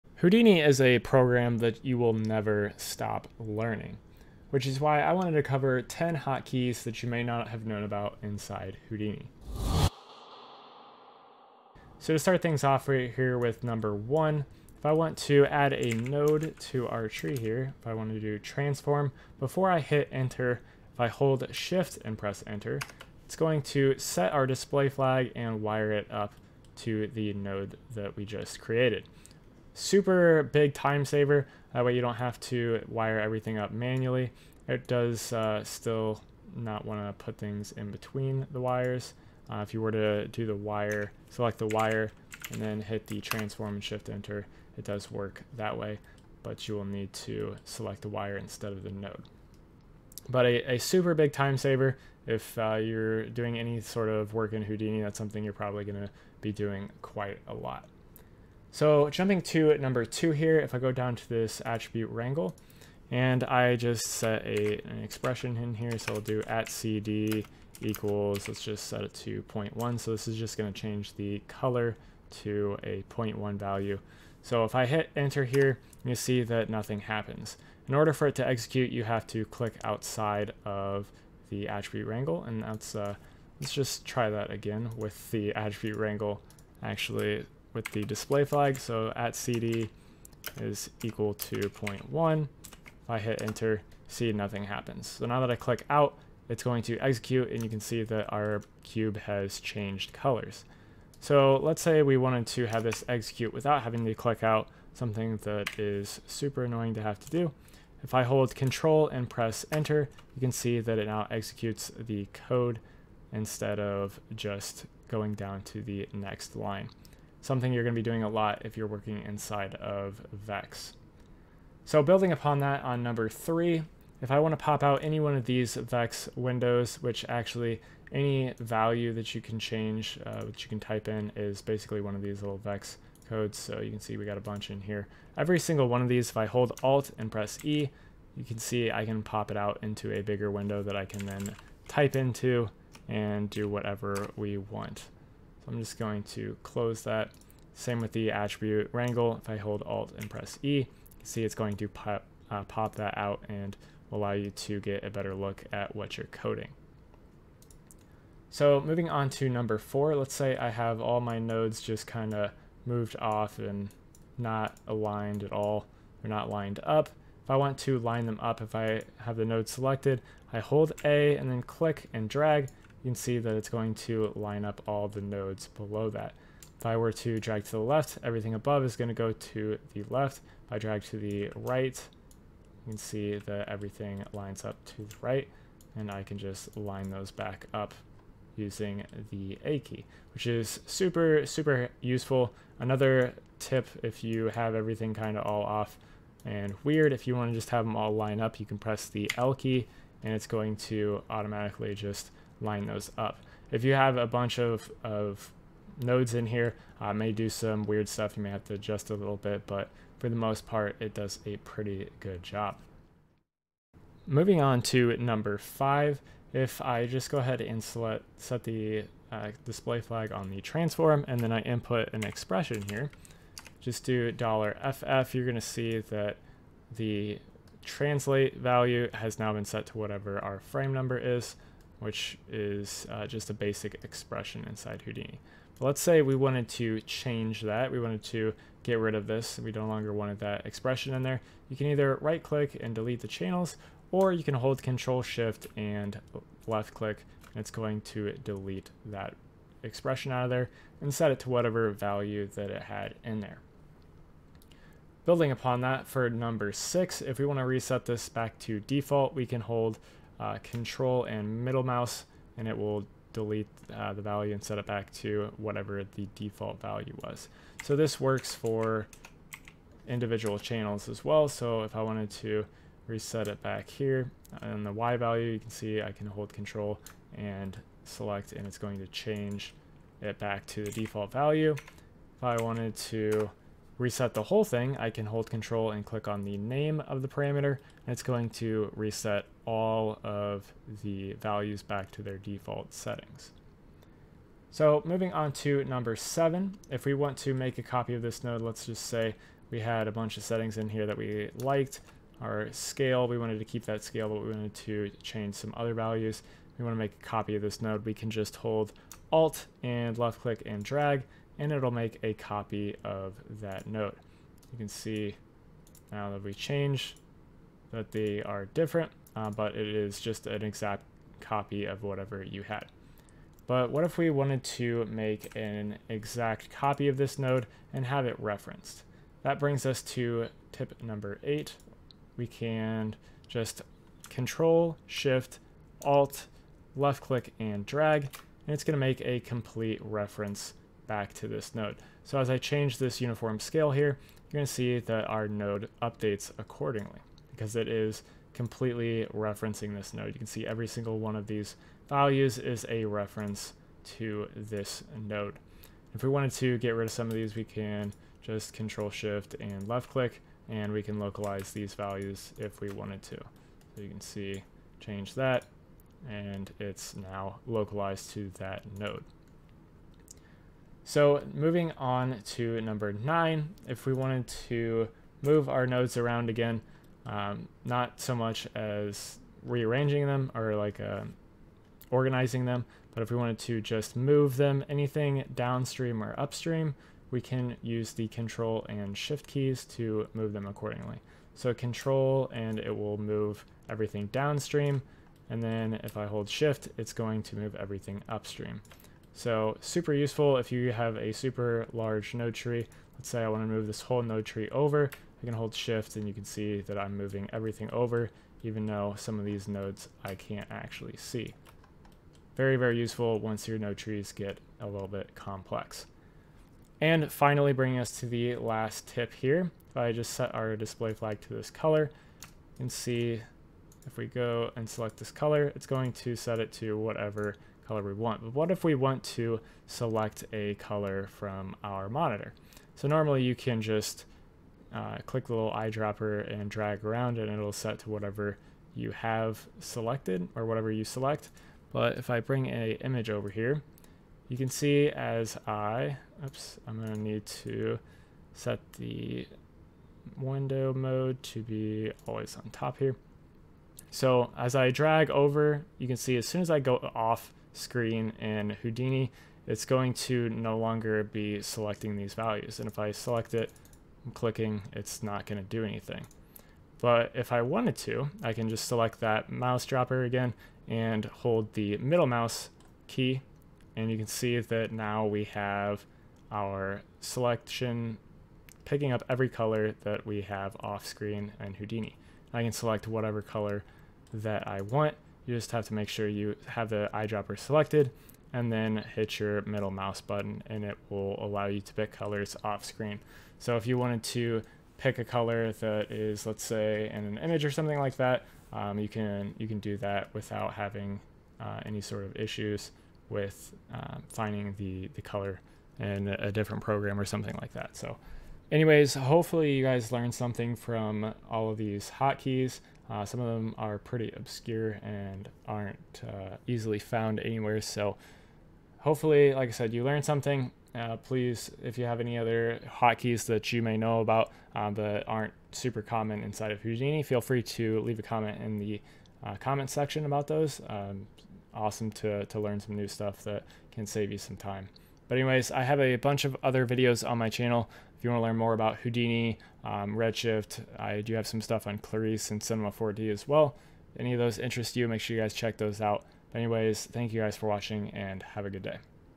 10 Houdini Hotkeys You Did Not Know
Houdini has a ton of hotkeys to make your life easier. A lot of these are pretty common and well known. There are some other hotkeys that are incredibly useful but not as well know. That is why we are taking a look at 10 Houdini hotkeys you may not know.

Please let me know what you thought of the video.